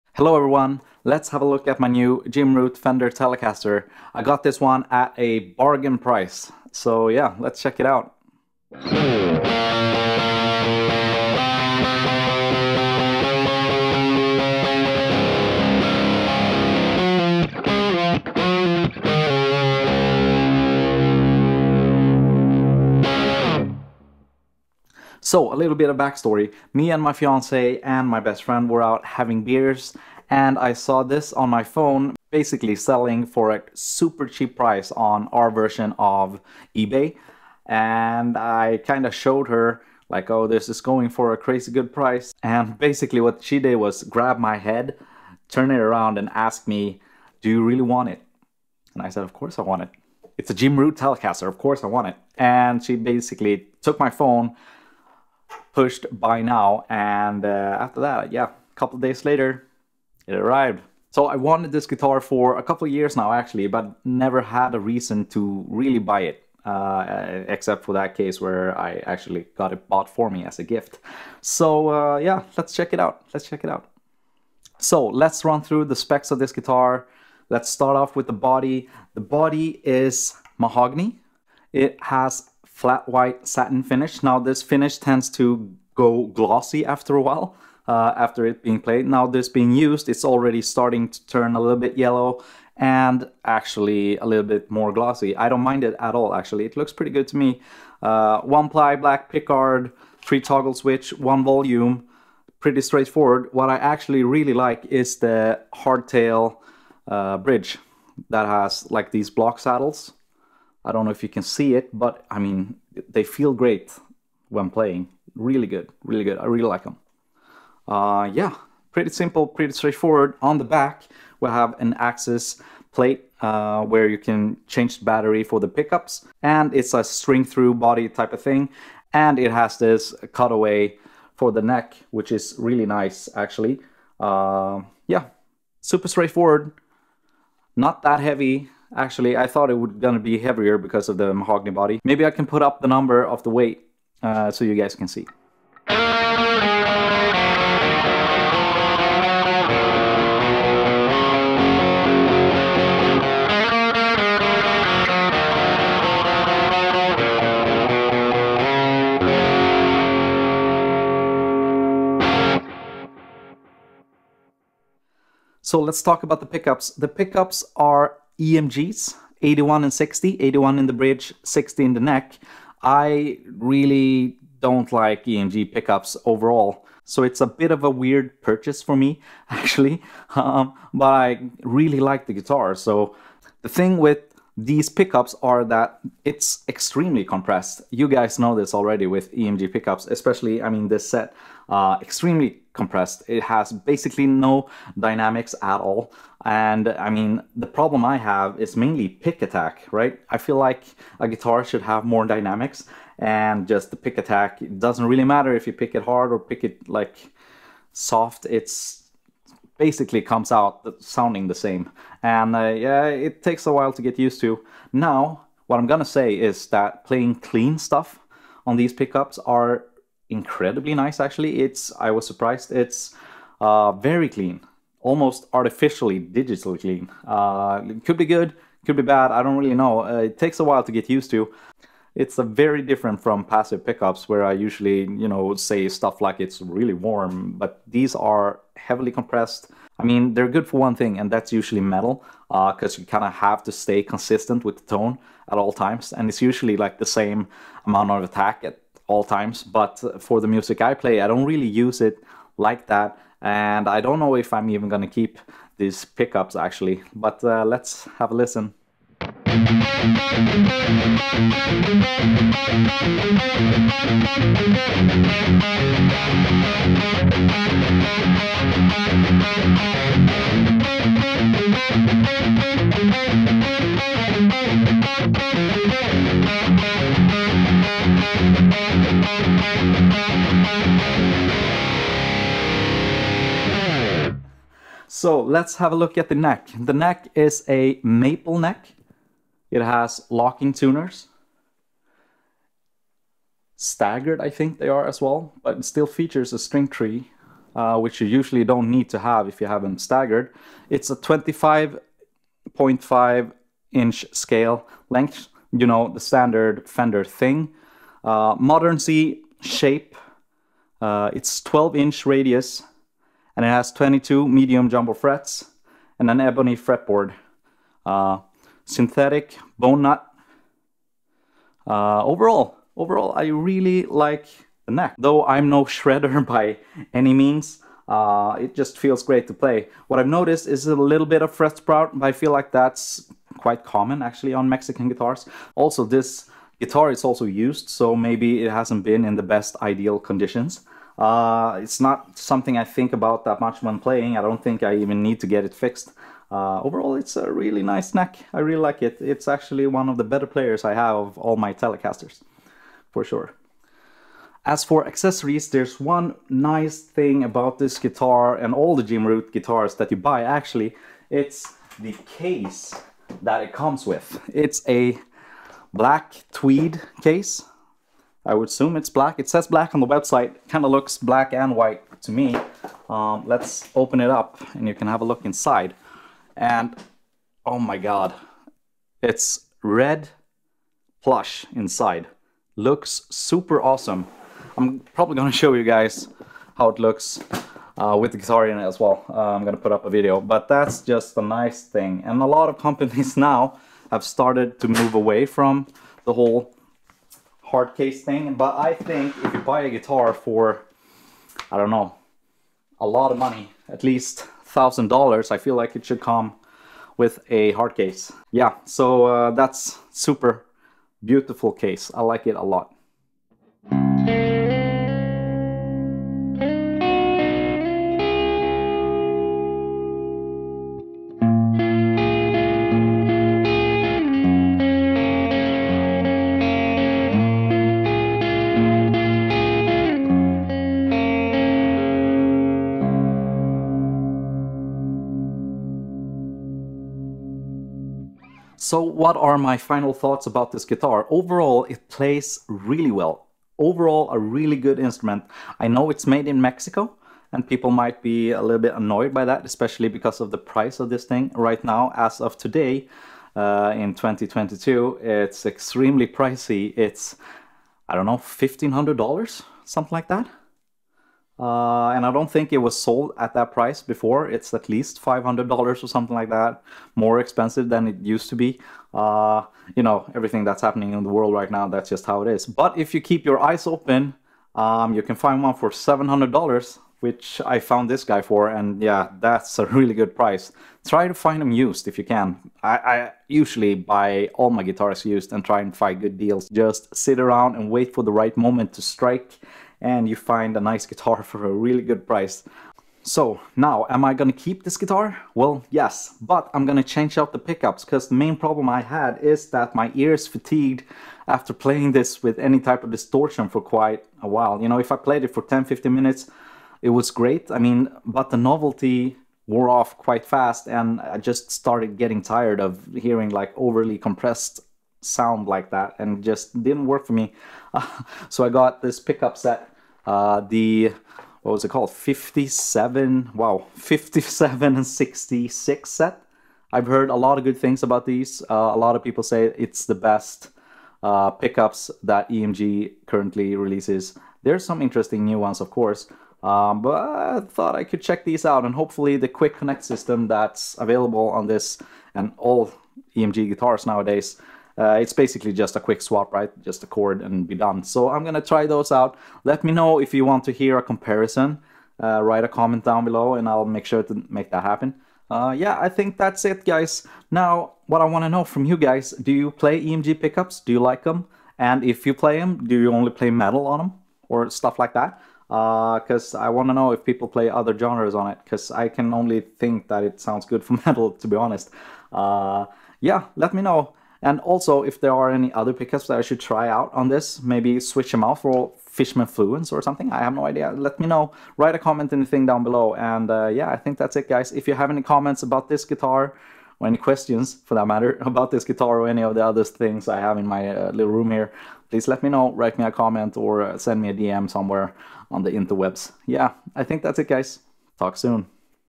Almost Perfect... The Jim Root Telecaster
I show you guys my new Fender Jim Root Telecaster in Flat White.

Time Stamps:
0:00 - Intro
0:08 - Sound Clip #1
0:25 - Story Time
1:14 - The Body
2:25 - Sound Clip #2
2:45 - The Pickups
4:19 - Sound Clip #3
4:45 - The Neck
5:54 - Accessories
6:53 - Sound Clip #4
7:19 - Final Thoughts

Audio Gear
Guitar: Fender Jim Root Telecaster. Flat White.
Amp: Kemper w. Bad Cat Panther, Bad Cat Lynx & Fender EVH 5153.
Interface: SSL2
Monitors: Yamaha HS7
Strings: Ernie Ball Paradigm 10-52

Camera Gear
Camera: Sony ZV-E10
Lens: Sigma 16mm f1.4 + Peter Mckinnon Variable Mist Filters
Audio: Rode Wireless Go + Rode Videomic NTG